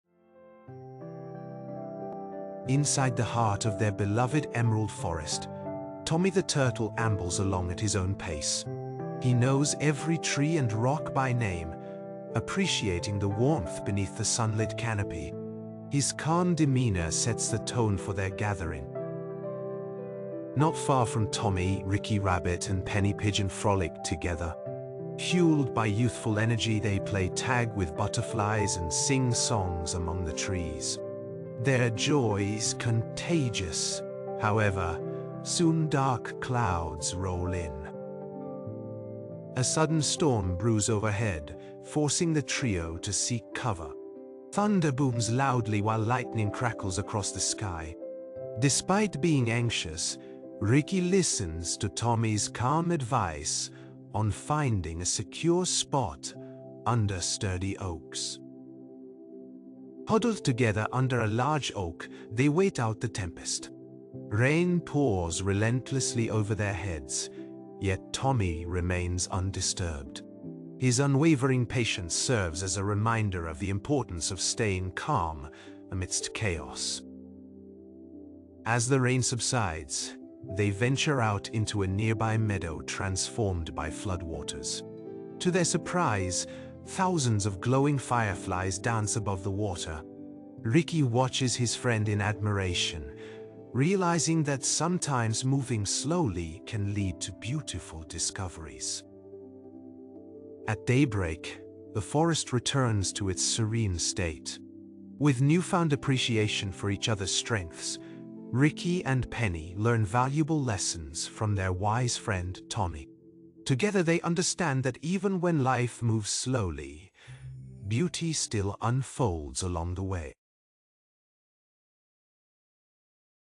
Tommy's Calmness: The Turtle Who Found Tranquility
Inside a lush emerald forest lives Tommy the turtle, who treasures every rock and tree in his home. Along with his friends Ricky the rabbit and Penny the pigeon, they enjoy carefree days exploring their surroundings. One day, as a sudden storm brews overhead, the trio takes refuge under an oak tree. Despite being anxious, Ricky finds comfort in Tommy's calm advice on finding safety. After waiting out the tempest, they emerge at dawn with newfound appreciation for each other's strengths and lessons from their wise friend. Through this experience, they understand that beauty still unfolds even when life moves slowly.